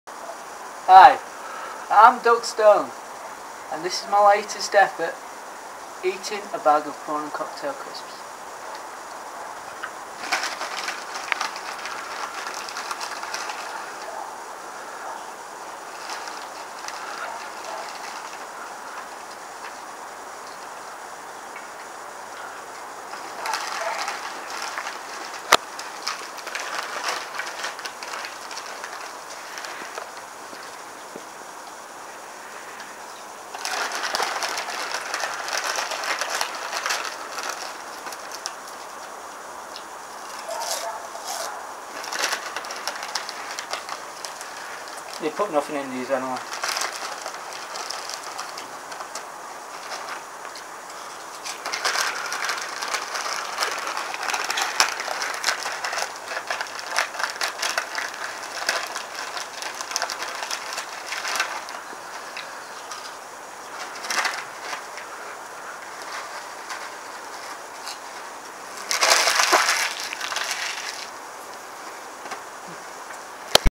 World of Wigan: Duck Stone - Eating a bag of Prawn Cocktail crisps
Where Duck Stone’s inspiration is mustered from is a total mystery to the good people at World of Wigan.

But his genius is unparalleled. And now he returns with his latest piece which is certain to smash through all boundaries of the currently accepted norm.

Warning: Prawn cocktail crisps are explicitly featured in this video.